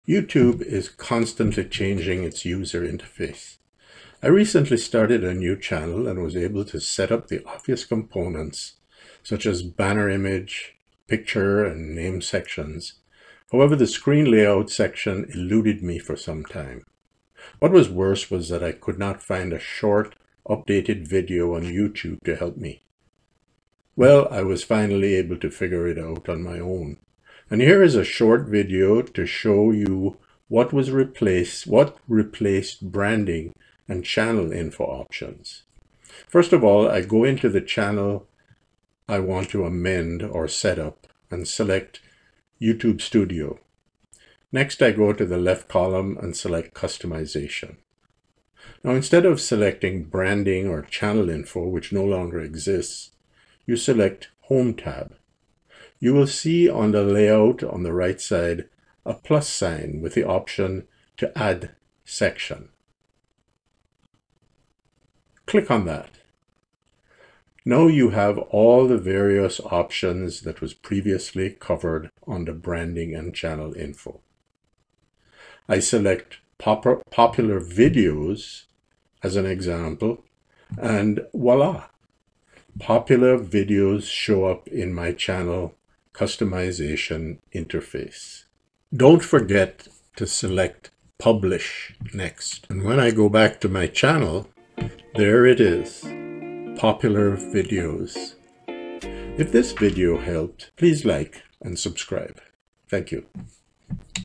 WHERE DID BRANDING & CHANNEL INFO options go - YouTube??
I recently started a new channel & was able to set up the obvious components such as Banner Image, Picture & Name sections, however the screen layout section eluded me for some time. What was worse was that I could not find a short, updated video on YouTube to help me.